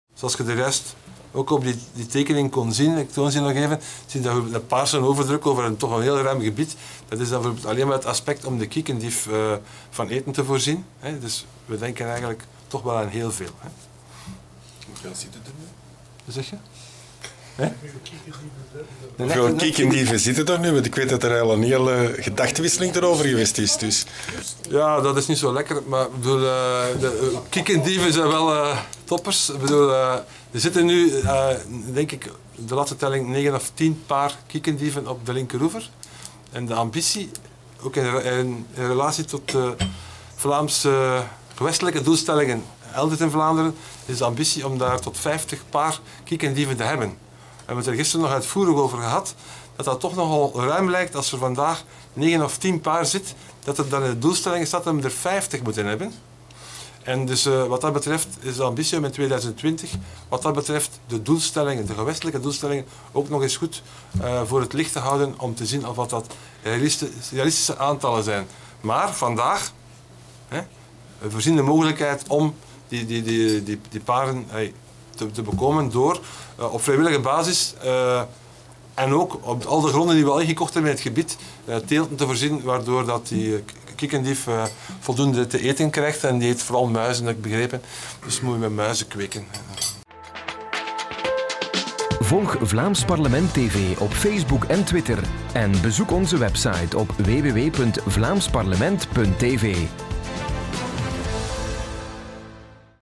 spraakmakers: "We moeten muizen kweken" - Freddy Aerts
Tijdens het plannen van de uitbreiding van de haven van Antwerpen om de containercapaciteit te vergroten, moet er met allerhande factoren rekening gehouden worden. Ook de kiekendieven in de natuurgebieden rond de haven worden beschermd en er wordt zelfs ruimte voorzien om de vogels van eten te voorzien.